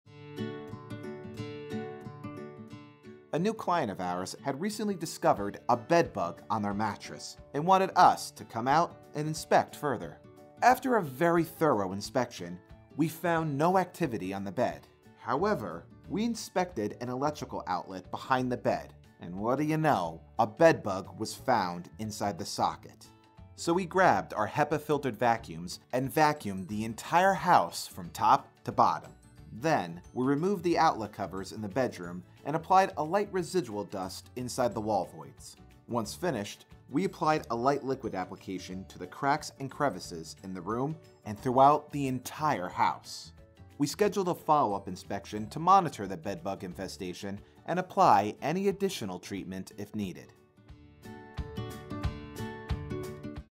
Hidden Bed Bugs in Long Branch, NJ
A new client of ours in Long Branch, NJ had recently discovered a bedbug on their mattress and wanted us to come out and inspect further.